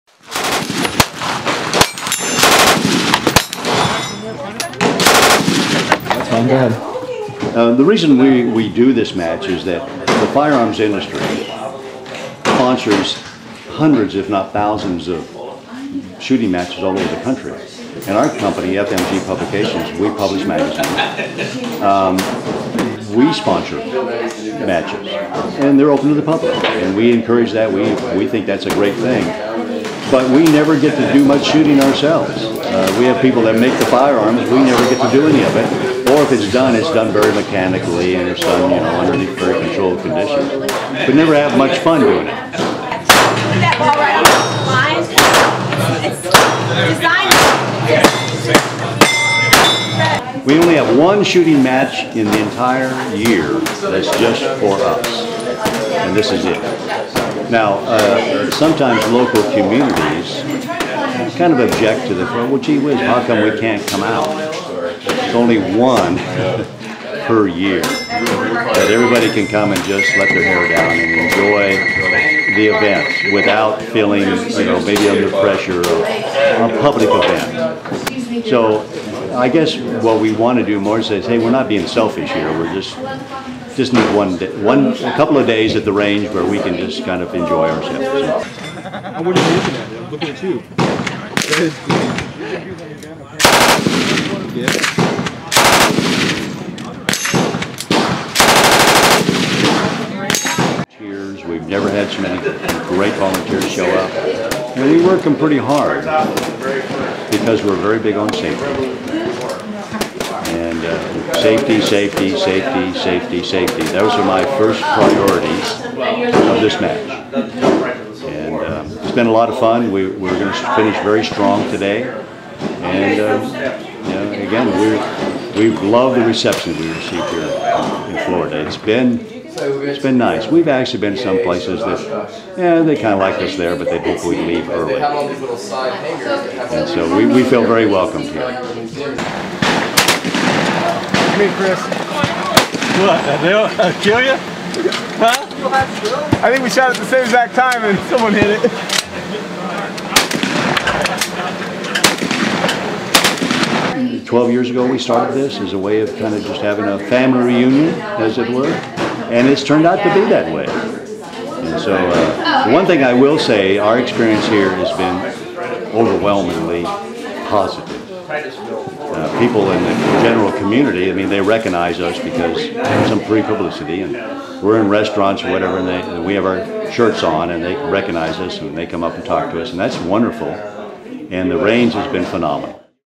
Shooting Industry Masters
Industry leaders come together in Indian River County at the shooting range for a two-day event. Staff video: Keith Carson. Read more and view photos at m.VeroNews.com